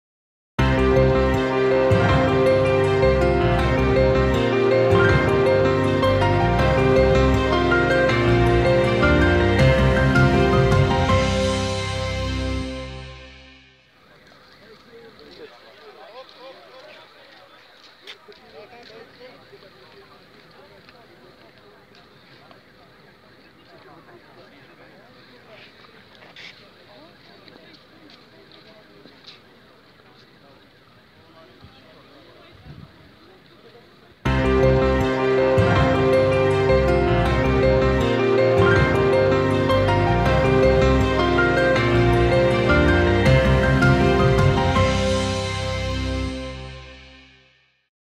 TLnews.ro | Copil dispărut pe lacul Ciuperca din municipiul Tulcea
Un minor aflat la scăldat la lacul Ciuperca a dispărut sub apă
VIDEO: ISU Delta Tulcea